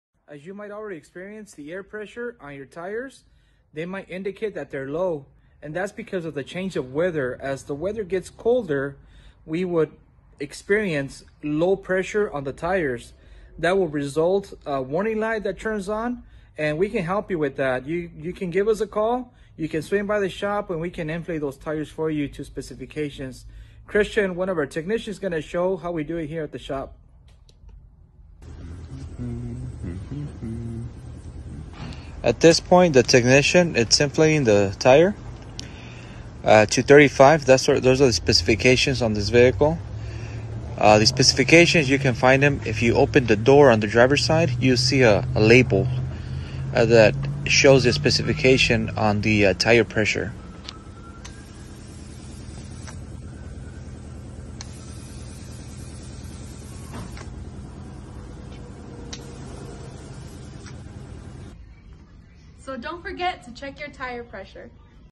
As we have changes in weather, we also have changes of pressure in our vehicles tires.
As we have changes in weather, we also have changes of pressure in our vehicles tires. When you bring your vehicle into our shop we will ensure that your vehicle has the proper tire pressure and inspect your tires for any possible concerns that we can resolve.